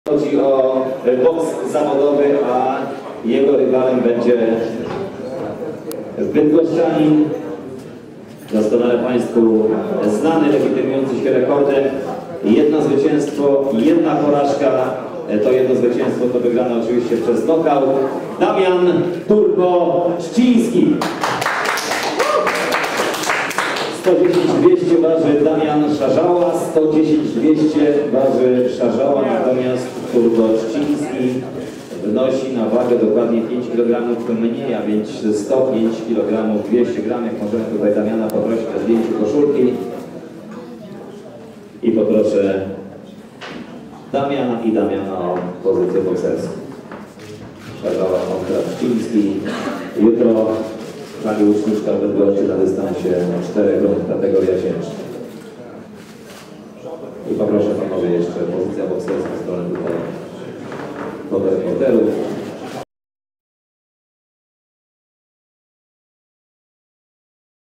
Oficjalne ważenie: Damian "Turbo" Trzciński vs Damian Sarzała
- Największy portal bokserski w Polsce przedstawia zapis video z ceremonii ważenia przed walką Damiana "Turbo" Trzcińskiego z Damianem Szarzałą.